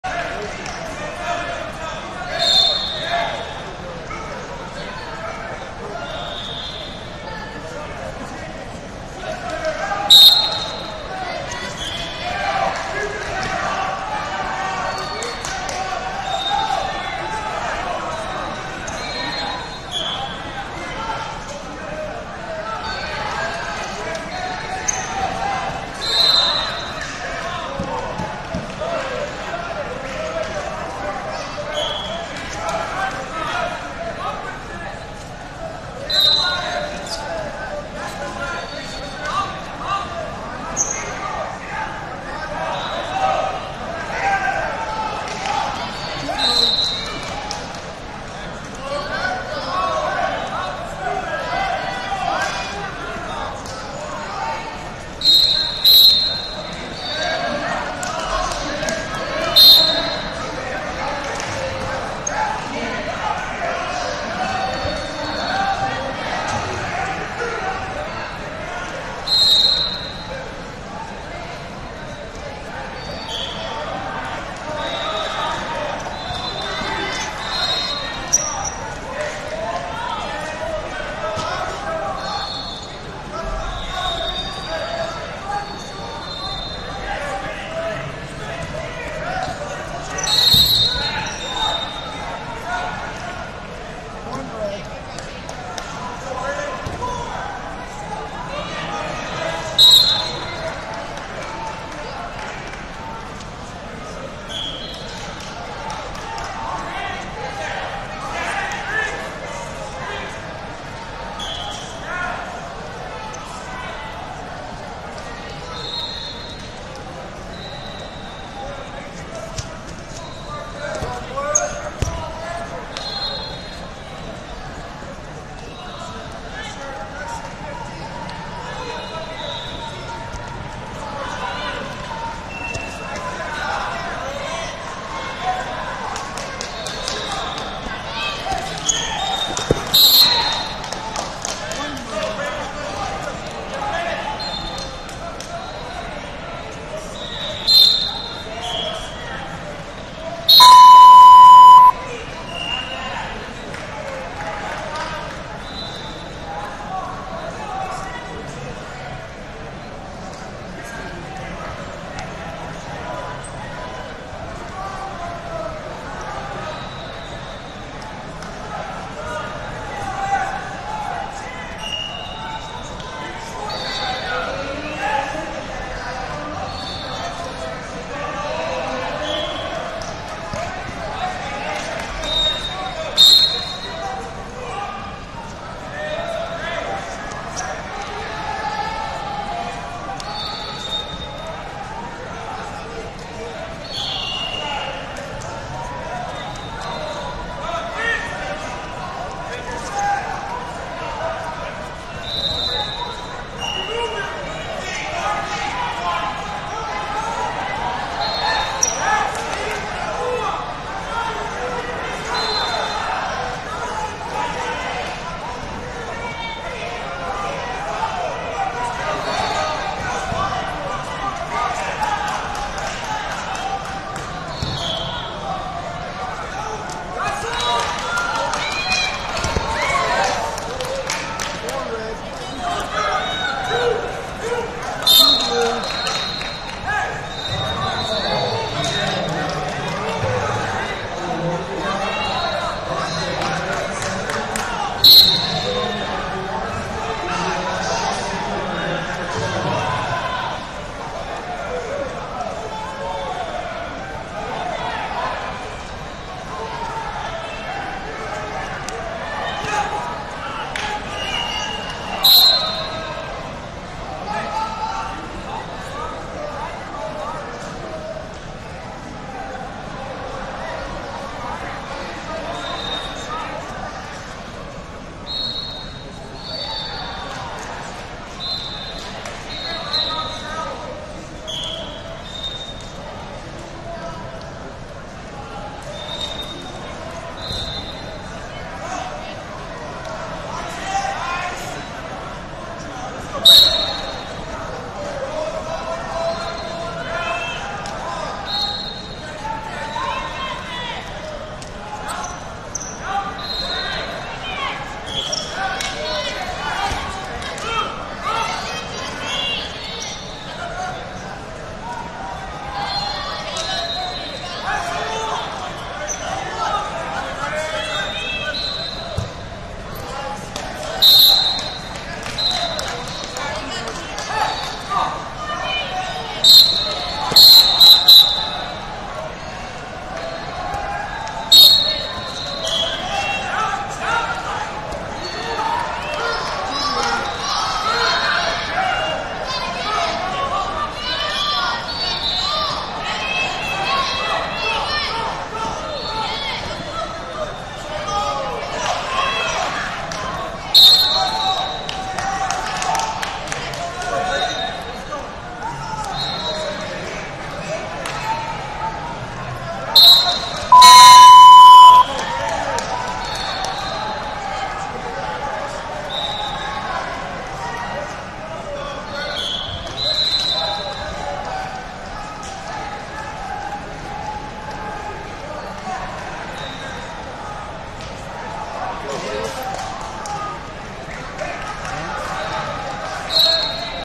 60 Kg Cons 16 #2 - Brayden Canoyer, MWC Wrestling Academy Vs Dawson Johnson, Wisconsin 9e22
Flosports-partner-access/2023 US Open Wrestling Championships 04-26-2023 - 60 Kg Cons 16 #2 - Brayden Canoyer, MWC Wrestling Academy Vs Dawson Johnson, Wisconsin 9e22